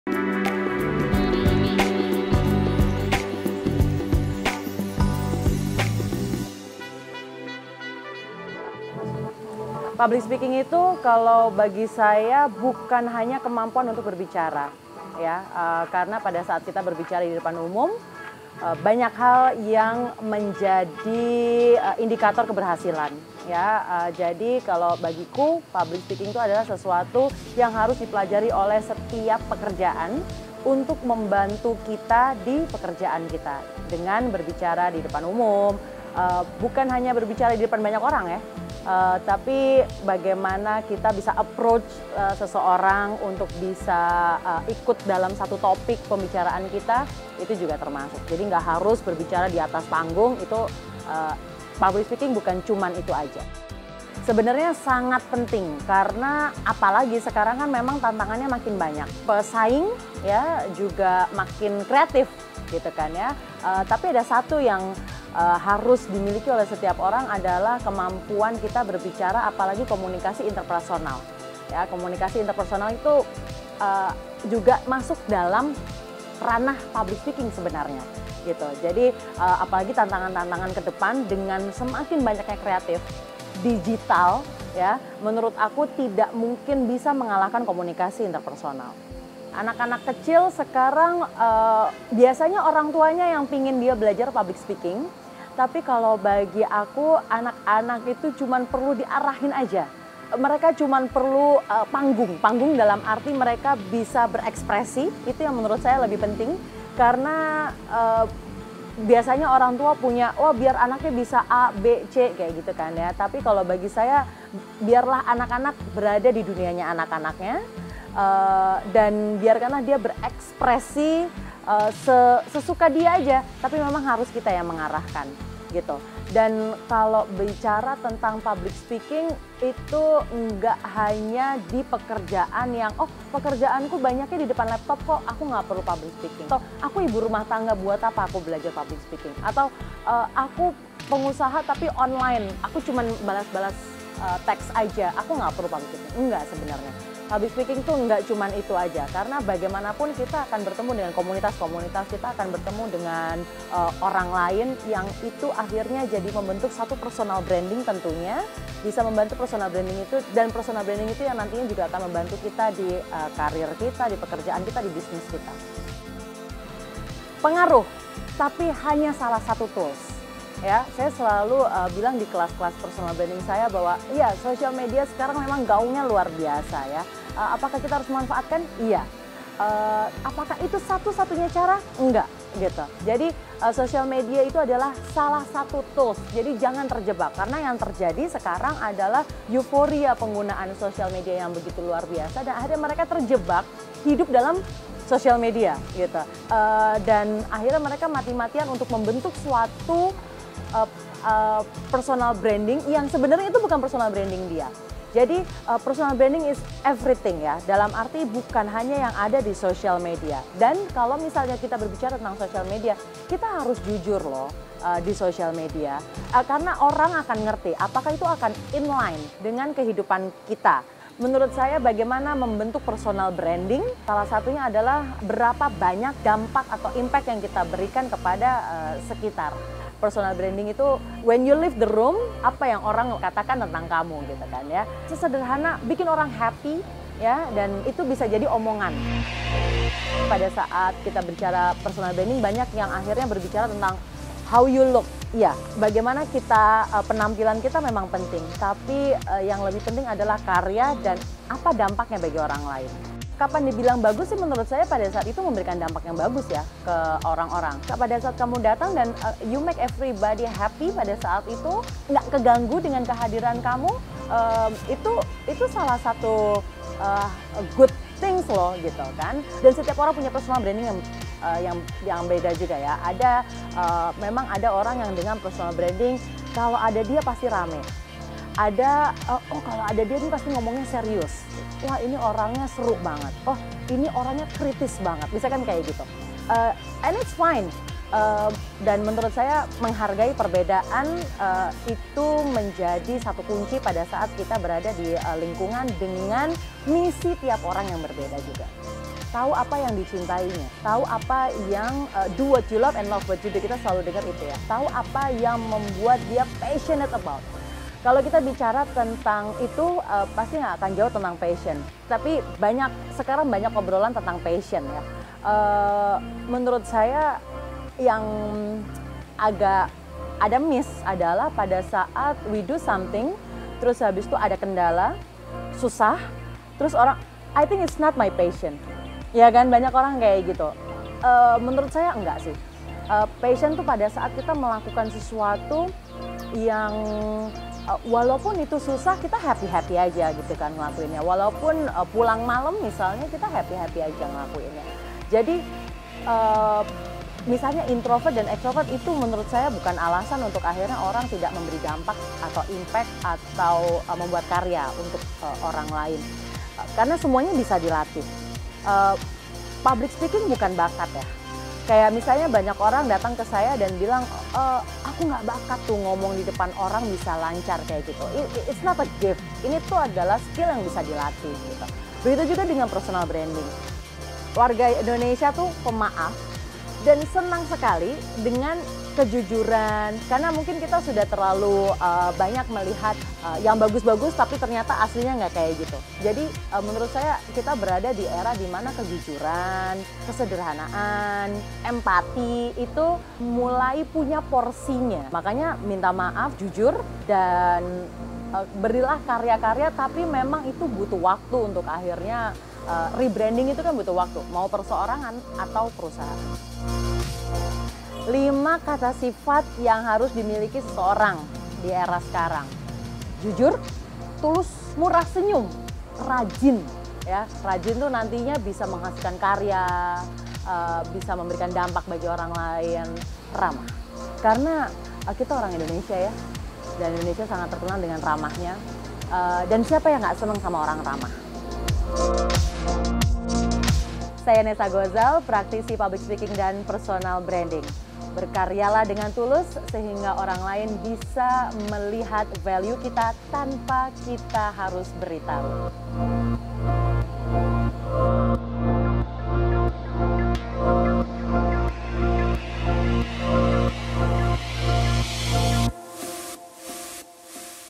Pentingnya Public Speaking Dalam Kehidupan Pribadi Maupun Pekerjaan - NET JATENG
Berkarya dan berdampak baik bagi orang lain, sangat diperlukan dalam kehidupan pribadi maupun pekerjaan. Karenanya, diperlukan sebuah kemampuan public speaking, untuk membantu pekerjaannya. Berikut paparan Nessa Gozal, praktisi public speaking dan personal branding dalam lembayung.